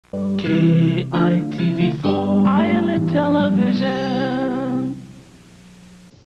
KITV (ABC) Station ID 1991 "Island Television"
This is KITV's station ID from 1991
KITV is located in Honolulu, HI